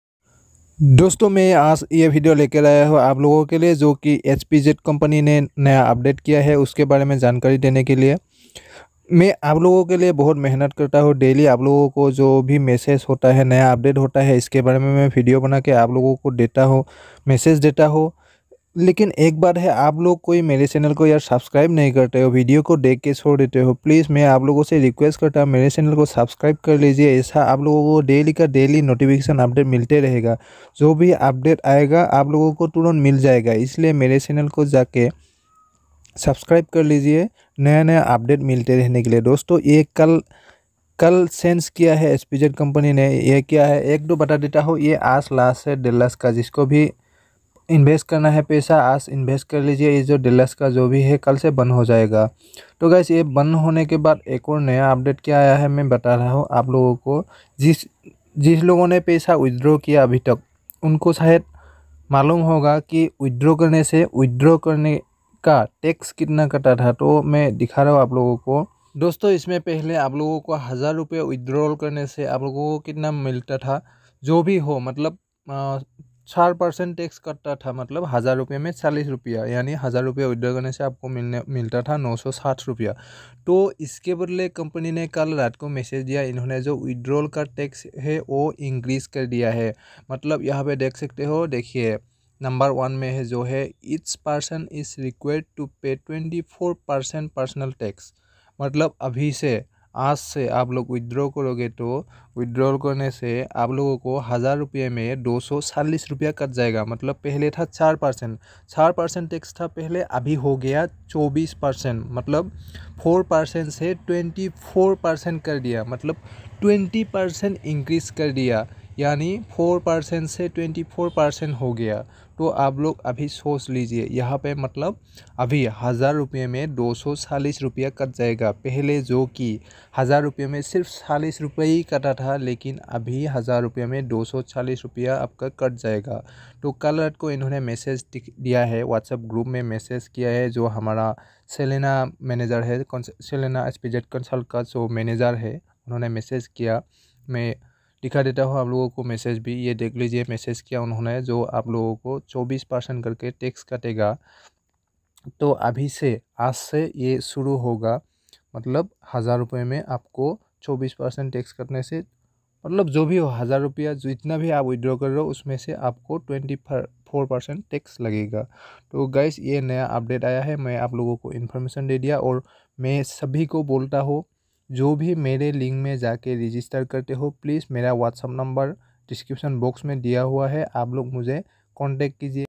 Hpz token Big update Money Withdrawal tax Increase 4% to 24% Oh my god
Hpz token money withdrawal tax increase 4% to 24% means if you withdraw 1000rs you will 960rs ll ArindamMix

📧Direct  massage to me Telegram